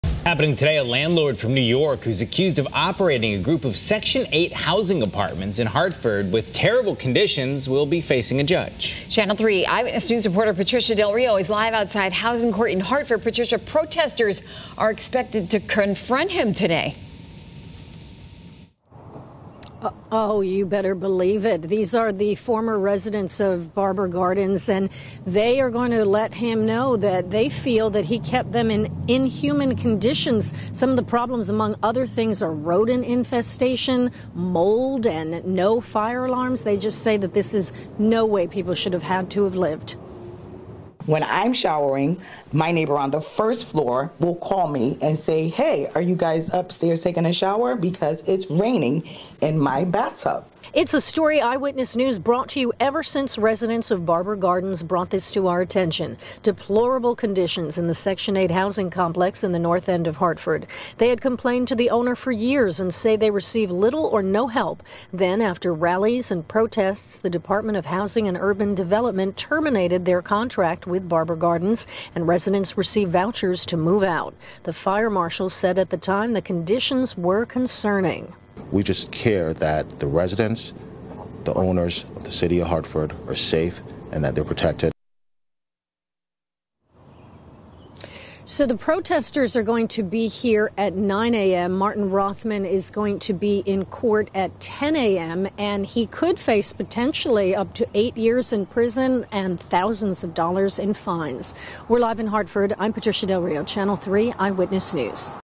VIDEO: Landlord set to face judge
A landlord of apartments in Hartford is set to face a judge on Tuesday after units were found to be in deplorable conditions.